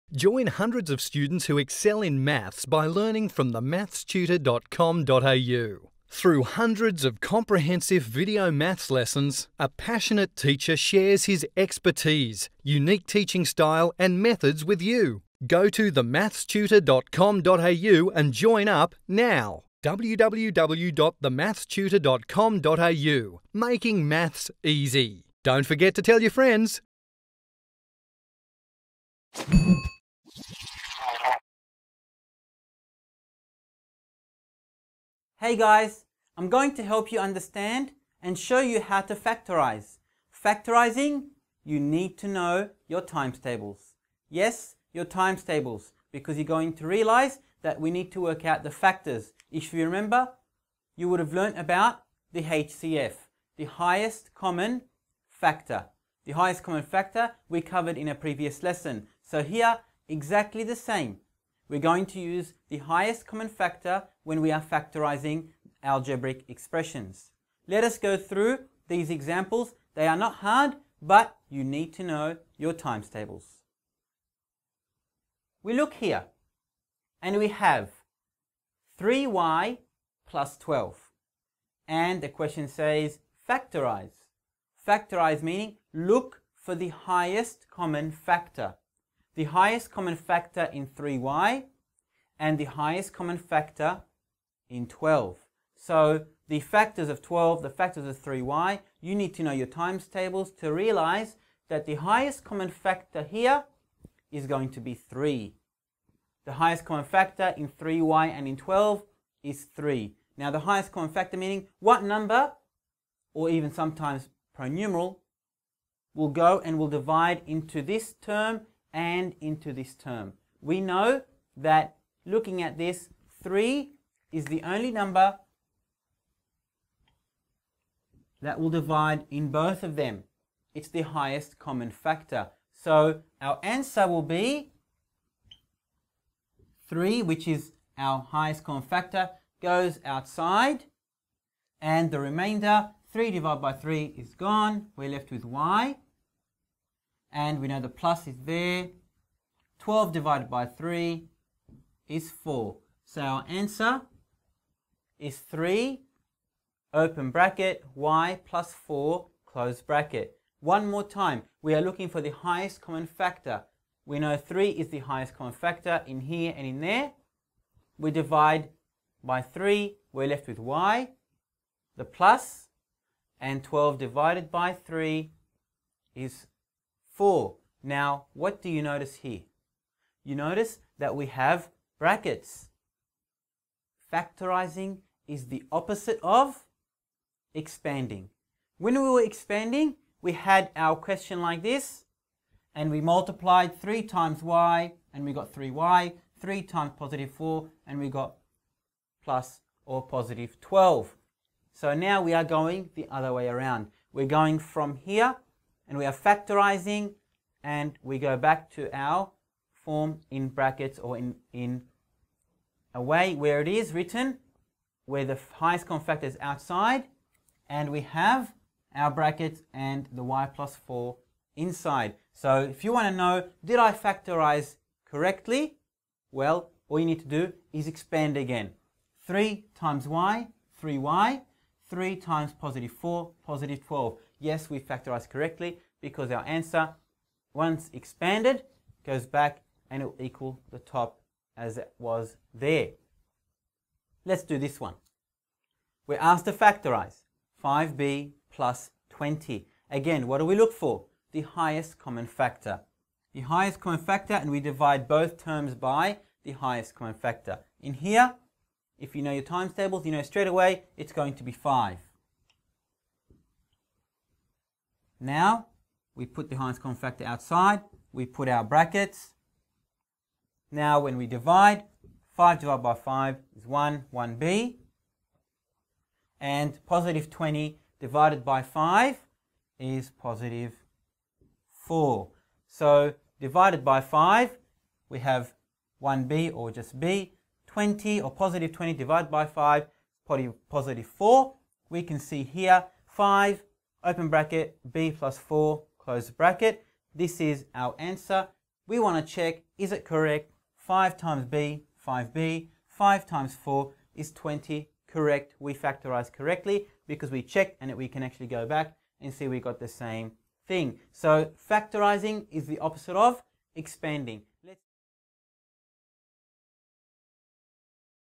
Maths Help Online - Factorising
"themathstutor" will teach you how to factorise algebraic expressions. Watch and Learn, its easy if you know your timestables!l